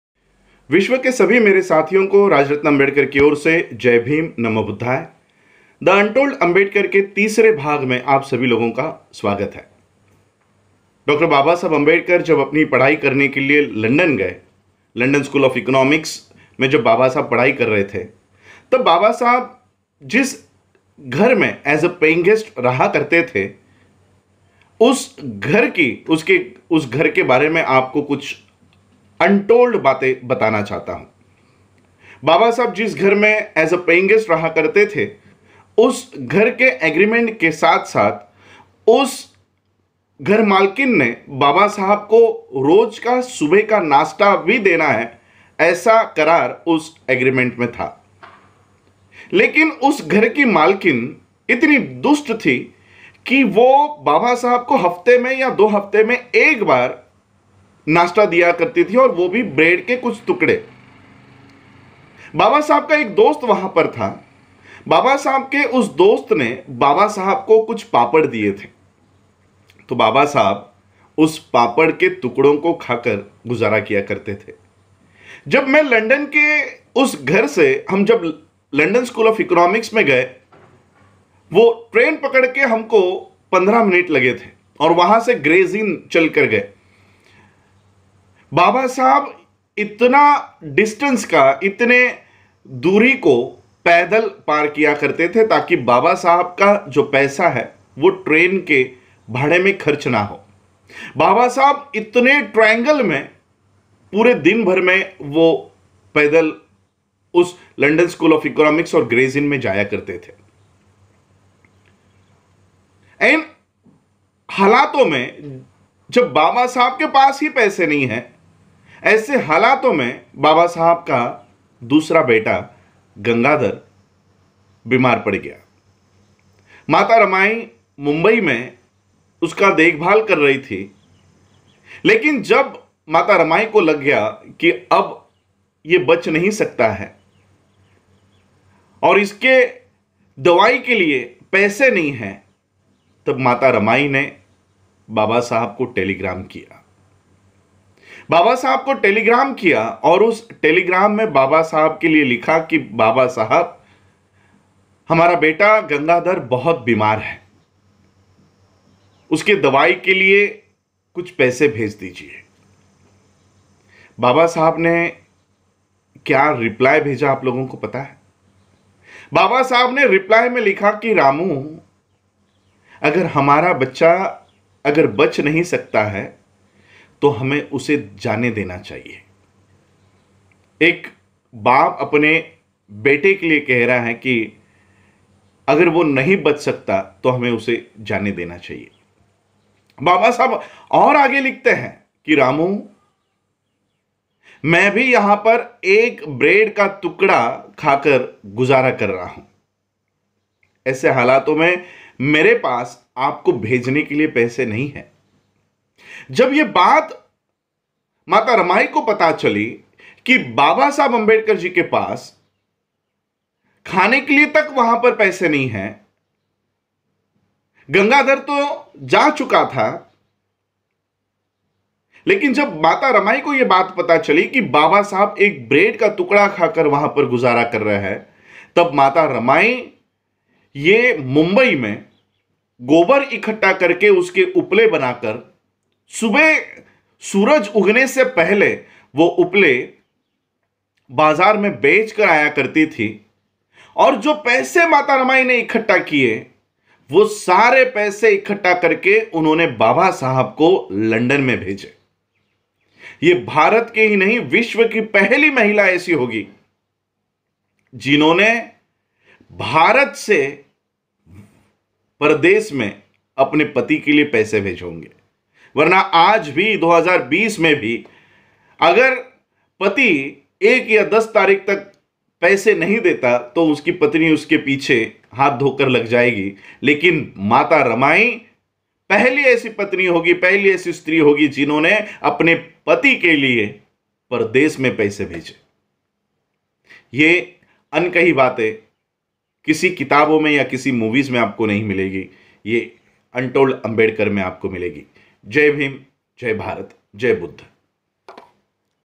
The Untold Ambedkar Episode 03
"जब माता रमाई को पता चला कि बाबासाहब लंदन में ब्रेड का एक टुकड़ा खा कर जी रहे है, तो जानिए माता रमाई ने क्या किया?"
The untold story of dr. B R Ambedkar - Rajratan ambedkar
डॉ. अम्बेडकर के कुछ अनकहे तथ्य, आपको उनके बारे में पता नहीं है।